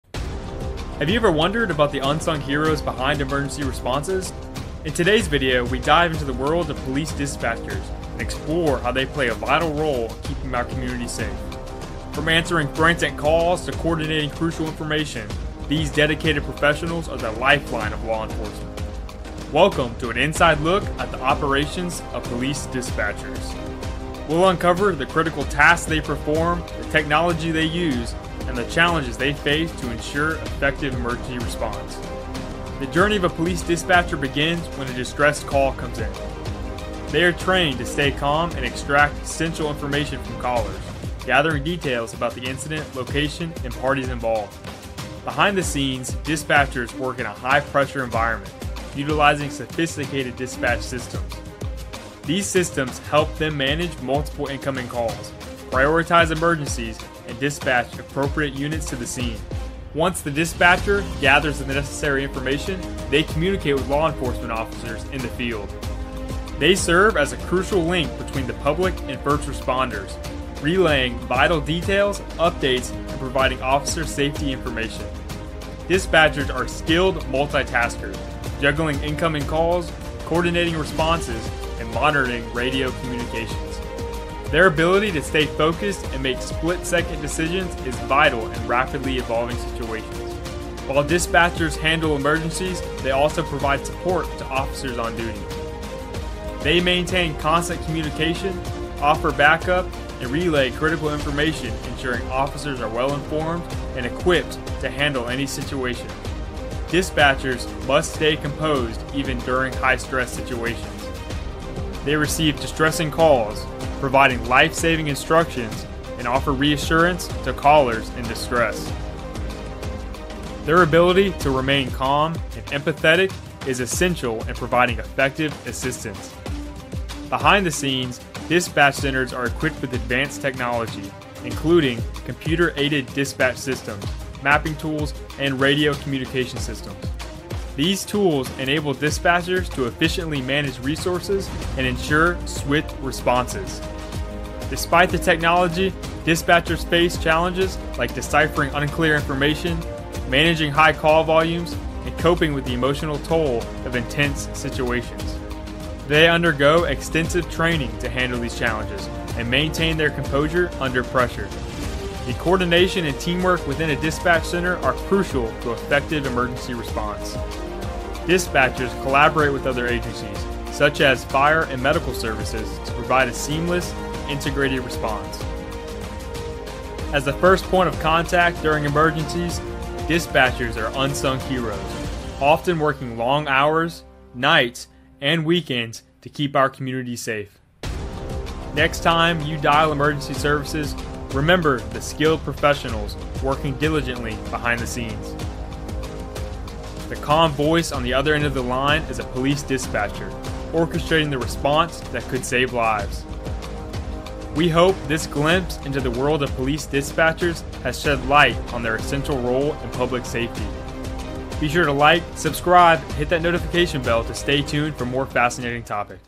What is a Police Dispatcher?
Have you ever wondered about the unsung heroes behind emergency responses? In this eye-opening video, we take you behind the scenes to explore the critical role of police dispatchers in ensuring public safety. From handling frantic calls to coordinating responses, these dedicated professionals are the lifeline of law enforcement.

Join us as we delve into the operations of police dispatchers, uncovering the essential tasks they perform and the technology they rely on. Discover how they gather crucial information from distressed callers, prioritize emergencies, and communicate with law enforcement officers in the field.

Witness the immense multitasking skills of dispatchers as they juggle incoming calls, coordinate responses, and monitor radio communications. Gain insight into the challenges they face, such as deciphering unclear information and managing high call volumes, all while maintaining composure under pressure.

Explore the advanced technology that powers dispatch centers, including computer-aided dispatch (CAD) systems, mapping tools, and radio communication systems. Understand how these tools help dispatchers efficiently manage resources and ensure swift responses to emergencies.

From providing lifesaving instructions to offering support to officers on duty, dispatchers play a crucial role in keeping our communities safe. Their calm demeanor, empathy, and ability to make split-second decisions are invaluable assets in the face of rapidly evolving situations.

Join us in acknowledging the dedication and commitment of these unsung heroes who work tirelessly behind the scenes. Gain a deeper appreciation for the vital role of police dispatchers in emergency response.